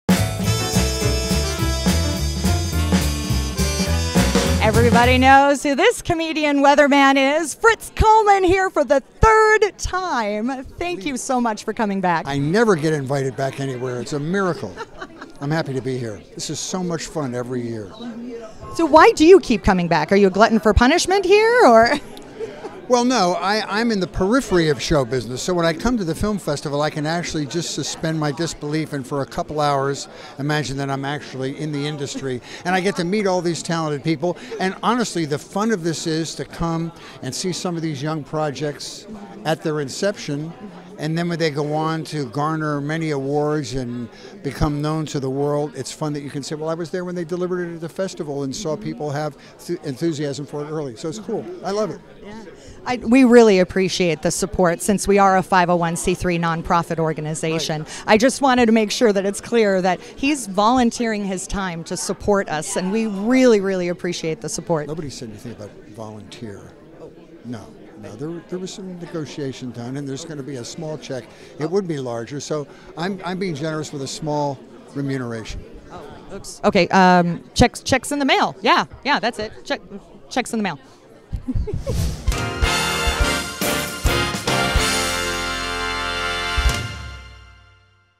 Red Carpet with Fritz Coleman
KNBC Weathercaster, Fritz Coleman
The Burbank International Film Festival